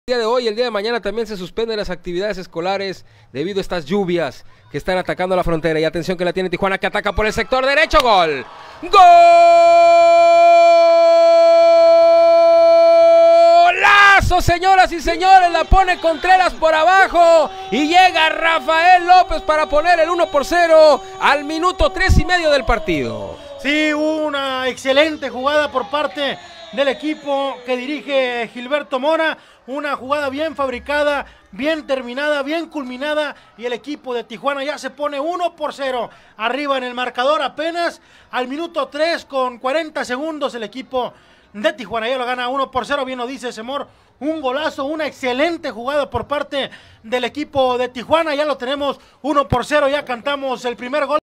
Gol 8 Rafa, Xolos vs Santos, Liga BBVA Sub17, 4tos de Final AP2019
Primer gol del jugador Rafael Lopez en cuartos de final, Marcador final Xolos 3 vs Santos 0